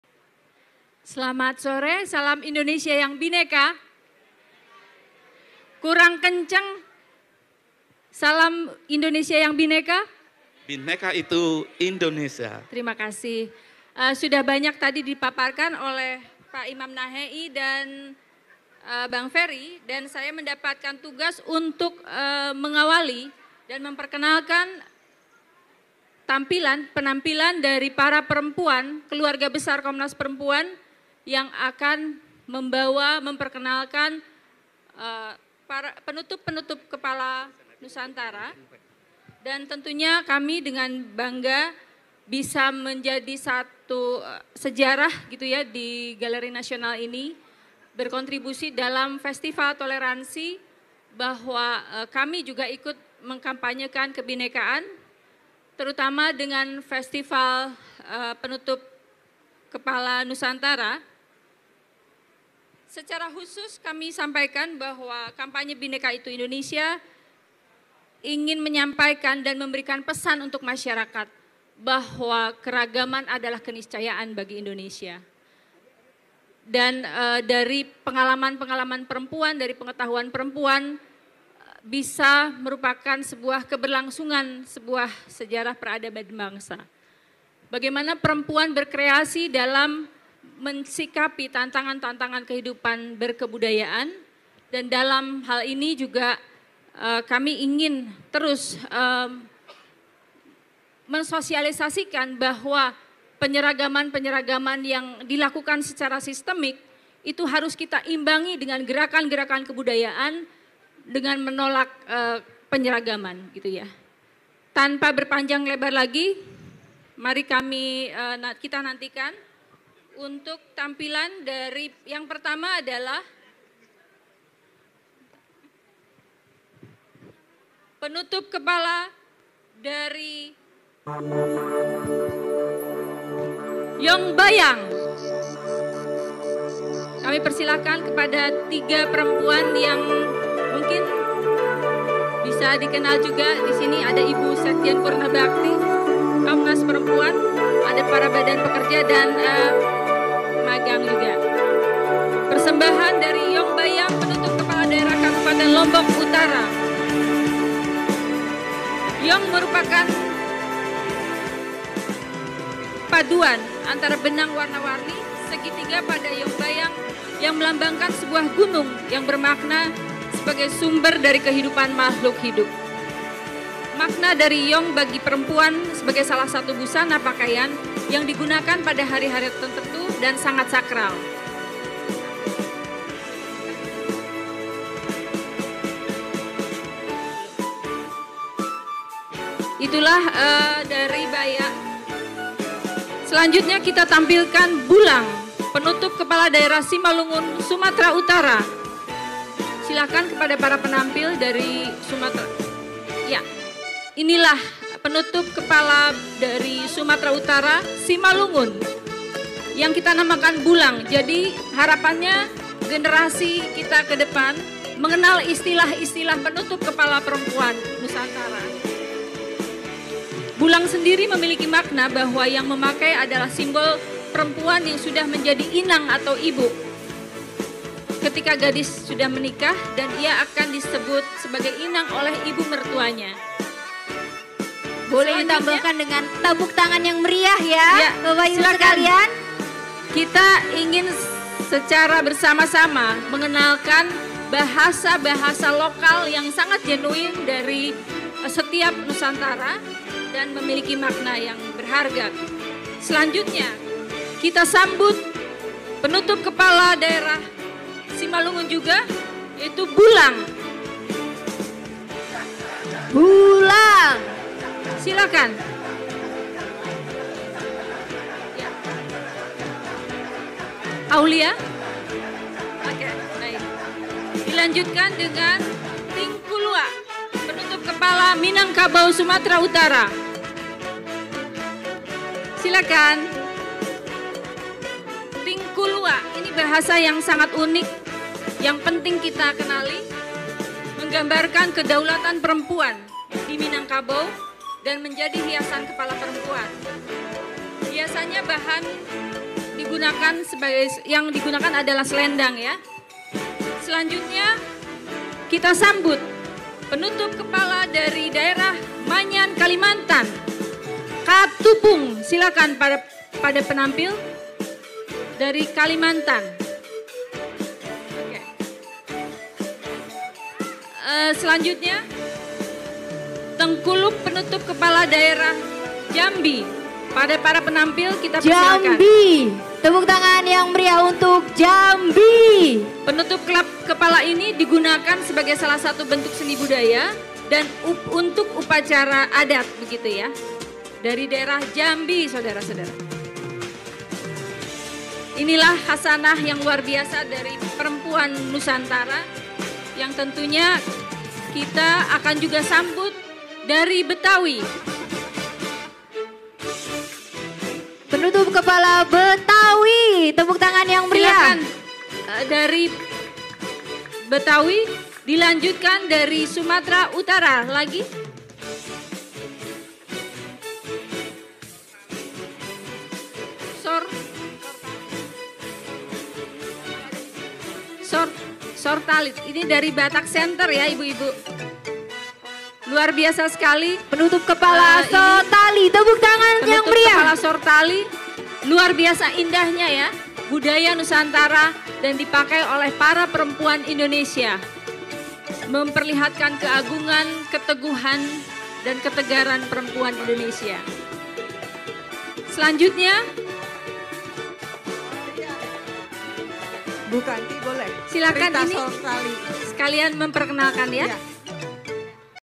Festival Toleransi Dewi Kanti
Festival Toleransi Hari kedua - P O P A R T M A R K E T

1. Lagu (Dina Miranda)
2. Puisi (Mahwi Ari Tawar)
3. Tari Sufi
4. Tari Betawi Cerite Kite (SMA Al-Izhar)
5. Fashion Penutup Kepala Perempuan (Komnas Perempuan)
6.Musik dan Lagu